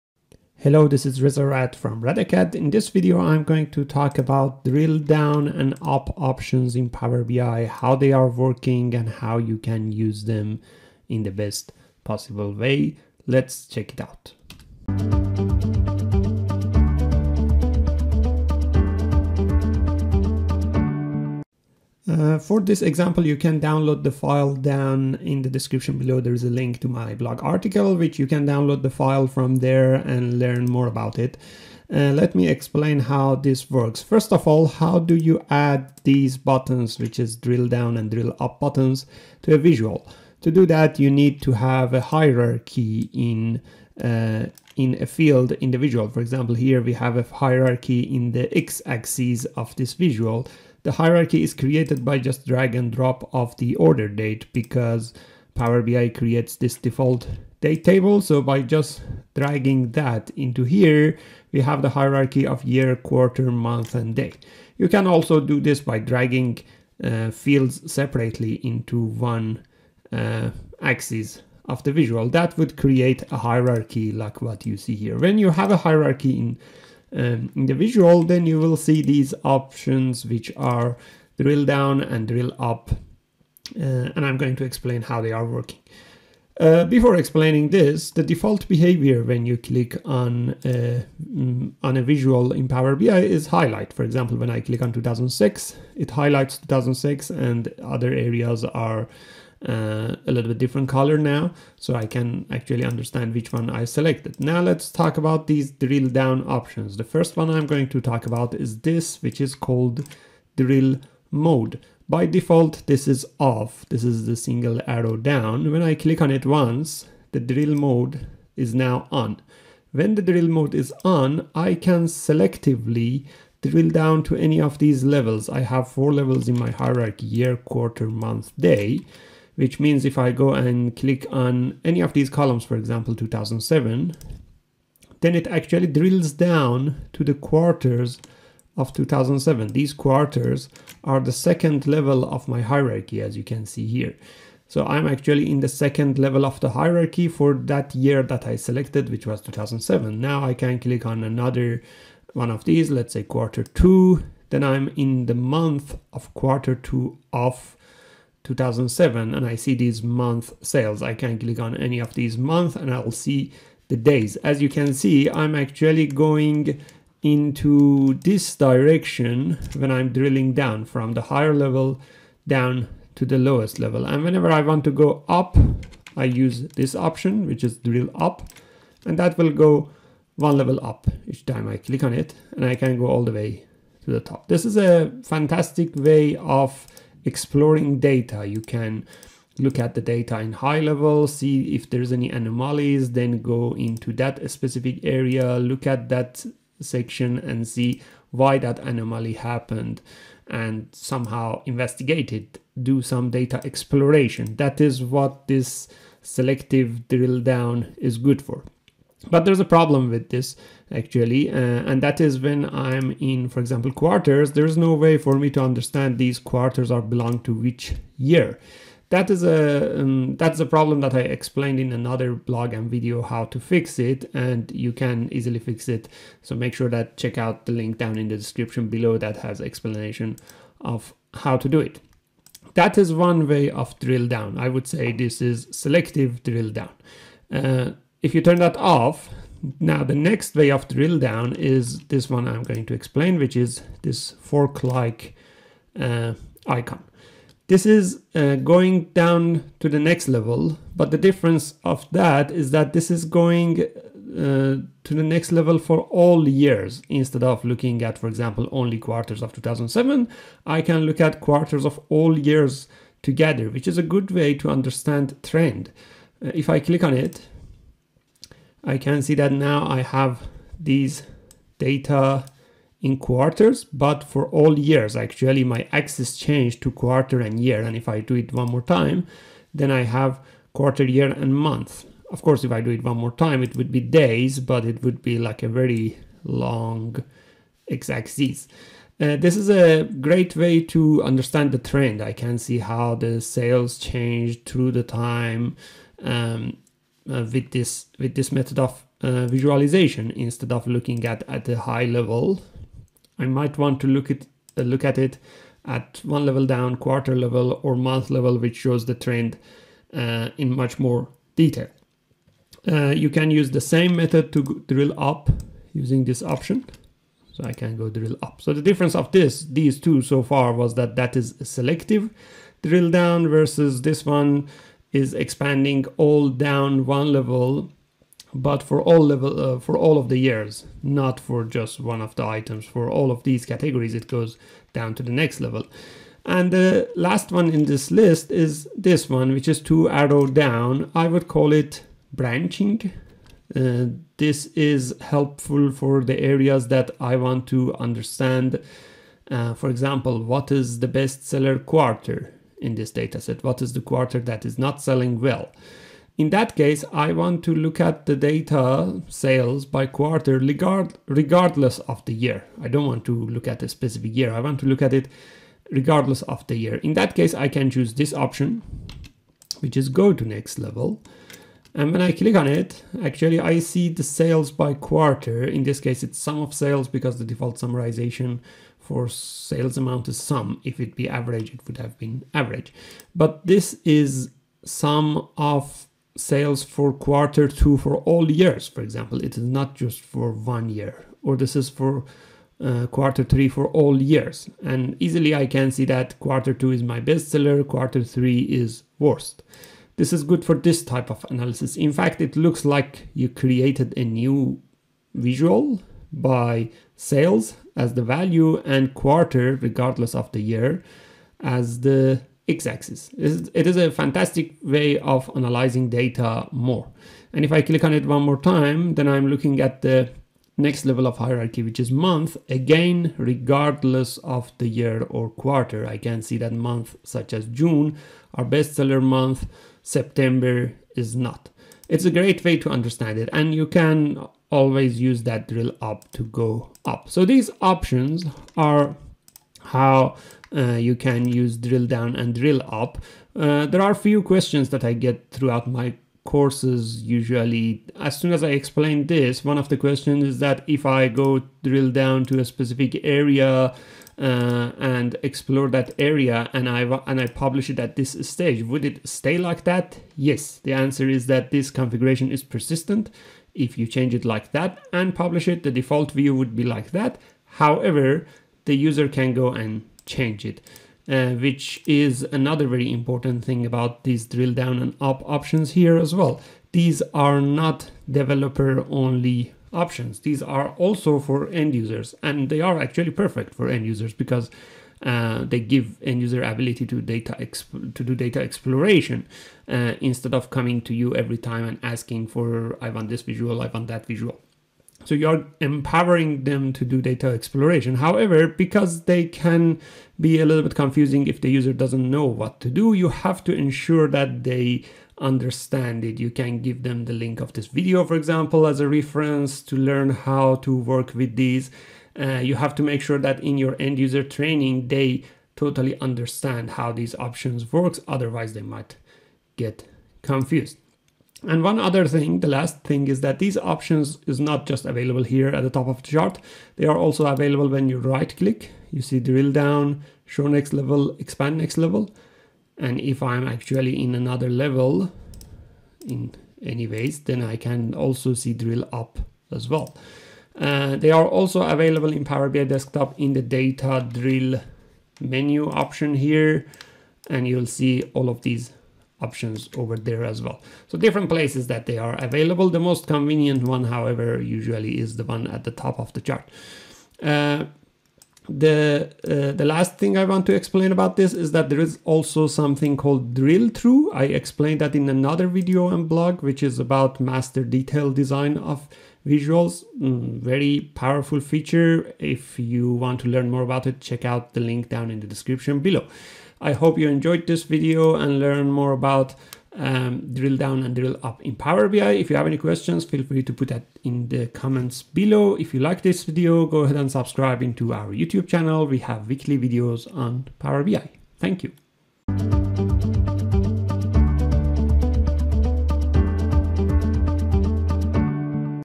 Drill Down and Up in Power BI Explained
Power BI works with hierarchies very easily, and there are buttons to drill down and up. However, sometimes, these buttons are confusing for the users. It is essential to know what each drill down/up option is and how it works, not just for developers but also for the end-users. In this blog and video, I’ll explain that.